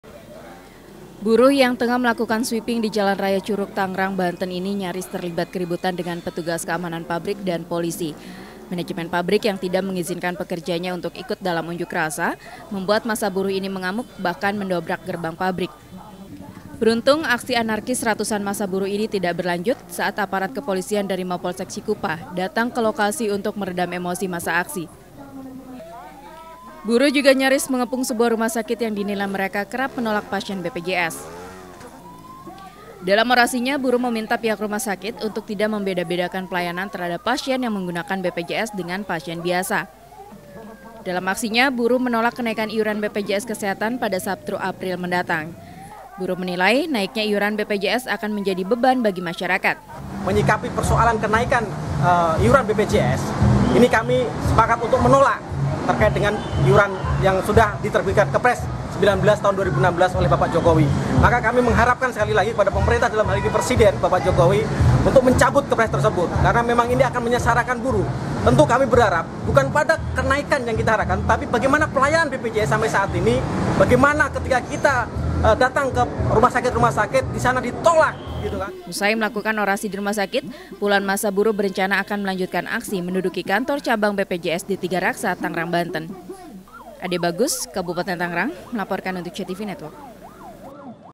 Unjuk Rasa Tolak Kenaikan Iuran BPJS, Ricuh
Kab. Tangerang, UNJUK rasa buruh menolak kenaikan iuran BPJS, di Jalan Raya Curug, Tangerang, Banten diwarnai keributan. Massa buruh yang akan menduduki kantor cabang BPJS melakukan sweeping, bahkan mendobrak gerbang pabrik untuk meminta pewakilan pekerja ikut dalam unjuk rasa, Rabu (23/3/16).